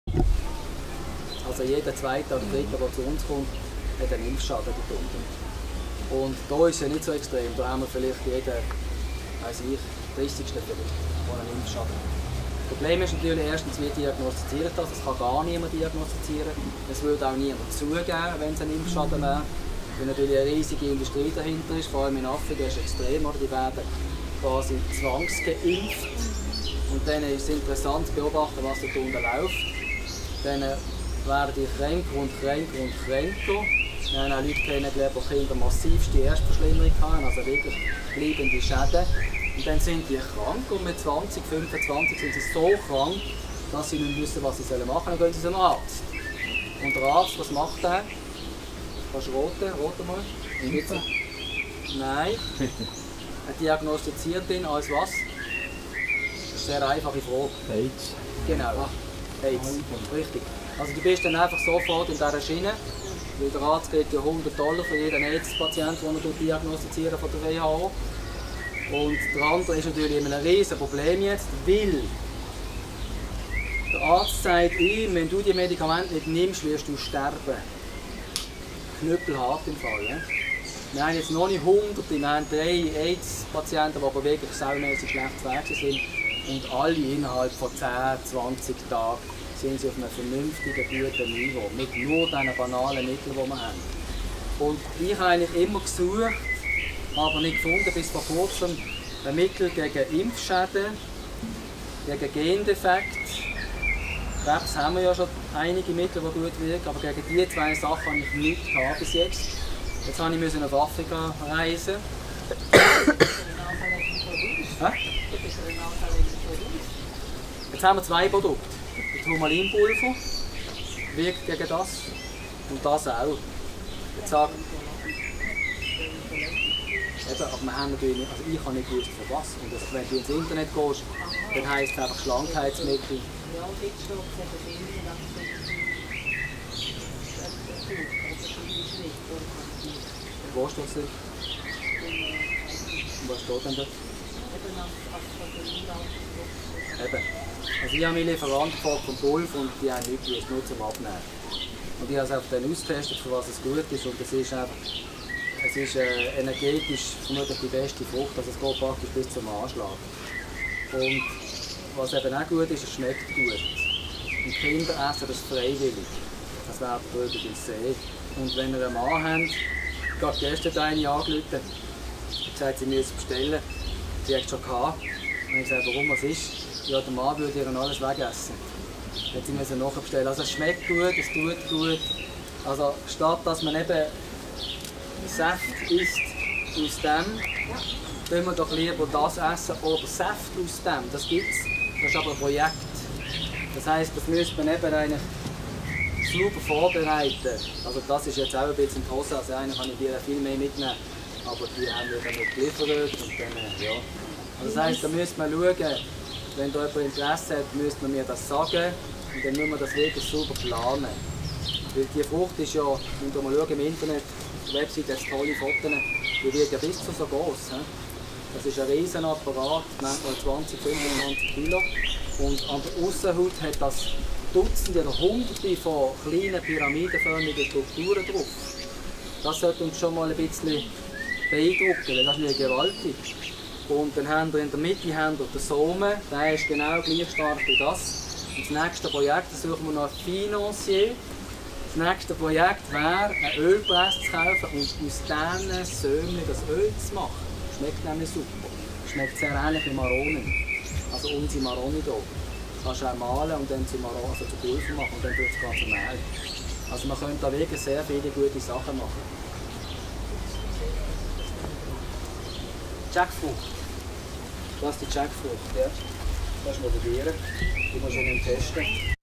Gesund durch Wissen, Zwangsimpfung, Impfschäden, Jackfrucht, Schweiz
Ein Vortrag von TATwellness, Andreas Schauffert in Bern, Schweiz.